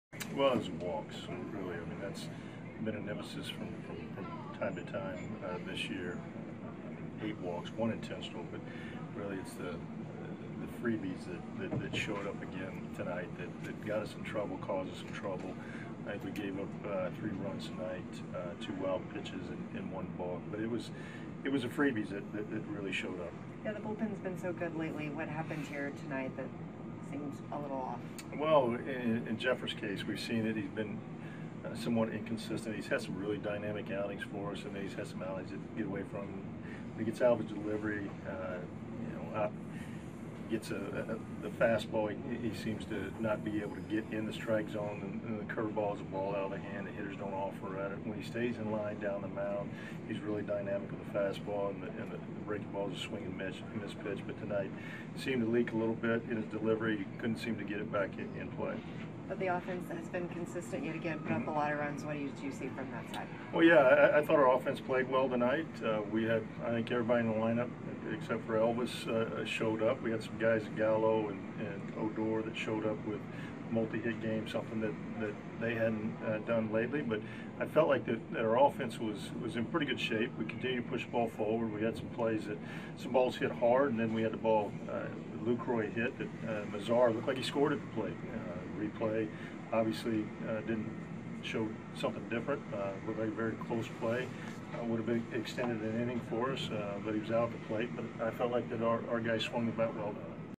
TEX@BOS: Banister on positives, negatives in the loss
Rangers manager Jeff Banister talks about Andrew Cashner's outing, the bullpen's struggle and the  recent strong offensive production